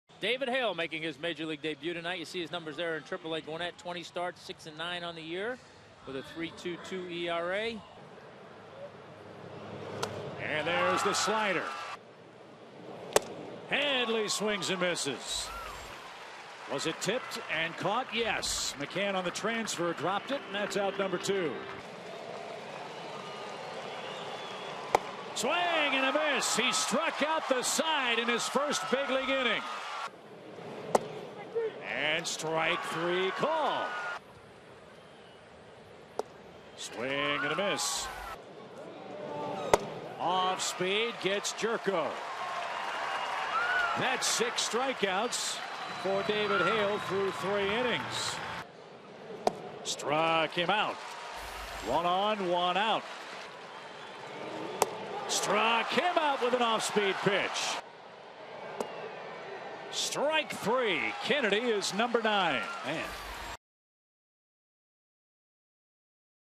2013/09/13 Hale fans nine in debut
SD@ATL: Hale fans nine over five scoreless in debut